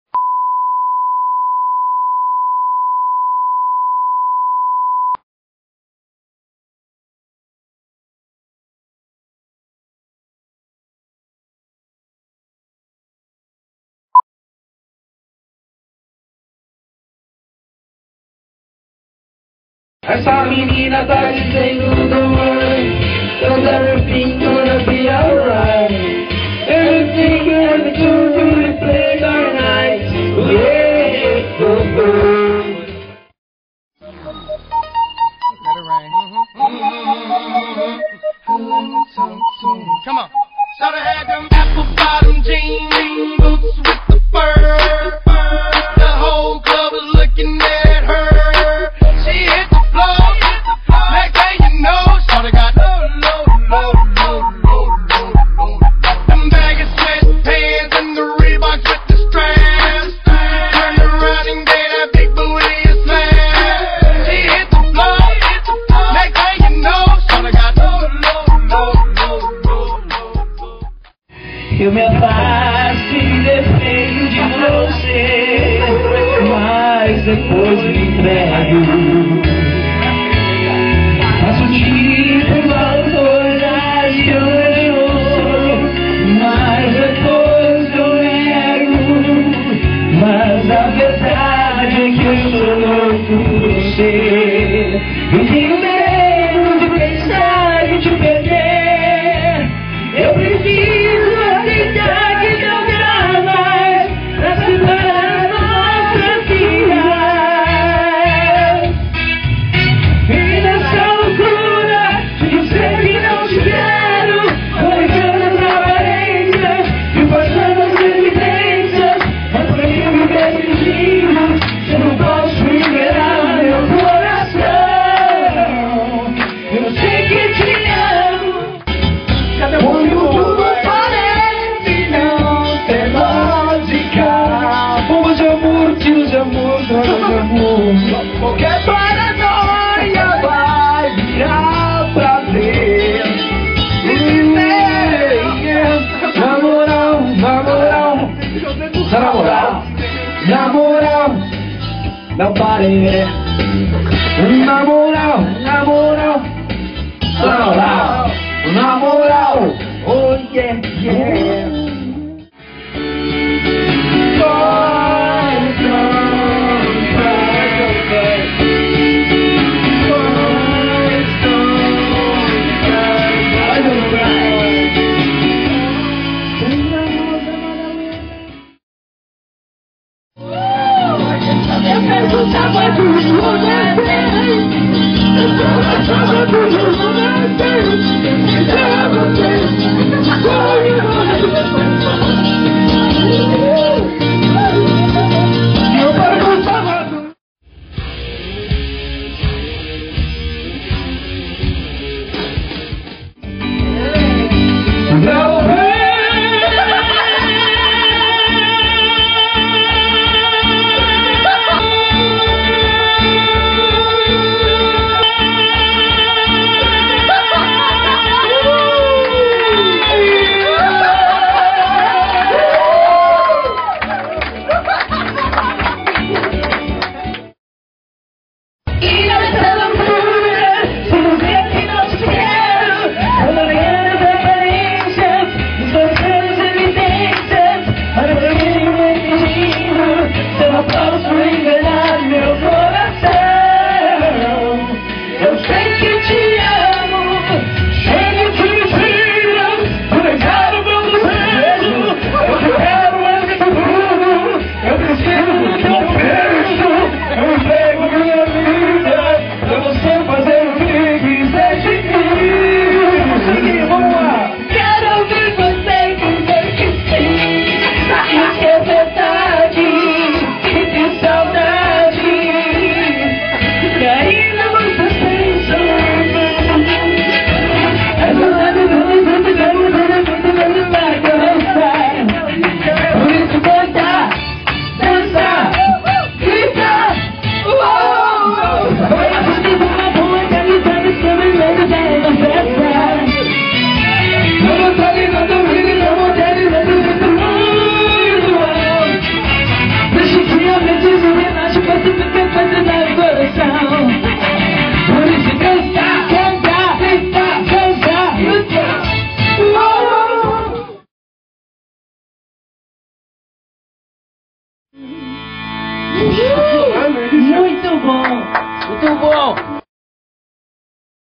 JBC no KARAOKÊ
A prova máxima de que um karaokê pode ser a balada mais divertida da sua noite, qualquer que seja ela. JBC e ex-JBC's unidos em uma só melodia (ruim).